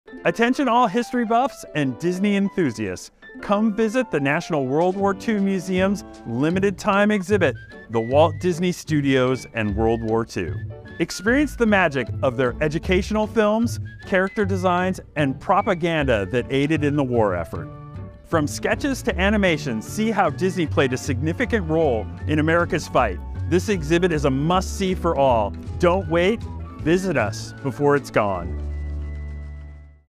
The Walt Disney Studios and World War II Special Exhibit at The National WWII Museum
Organized by The Walt Disney Family Museum in San Francisco, California, The Walt Disney Studios and World War II will be on display in The National WWII Museum’s Senator John Alario, Jr. Special Exhibition Hall from March 17, 2023, through September 24, 2023. This family-friendly exhibit explores how one of America’s most beloved entertainment companies helped achieve Allied victory.